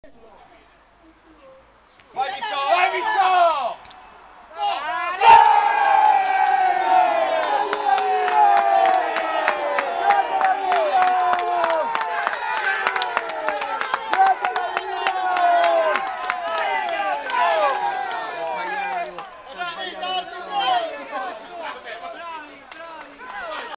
Campionato dilettantistico.Di seconda categoria del Lazio girone D (VT) Civitella- Shot Cassia.
Sport dilettantistico.Nel pomeriggio di oggi, alle ore 15,00 presso il campo sportivo di Capena (Roma) si e disputata la partita di calcio, di seconda categoria girone D (VT). Tra Civitella - Shot Cassia. Il Civitella agguanta la vittoria al novantesimo. Risultato finale Civitella 1 Shot Cassia 0.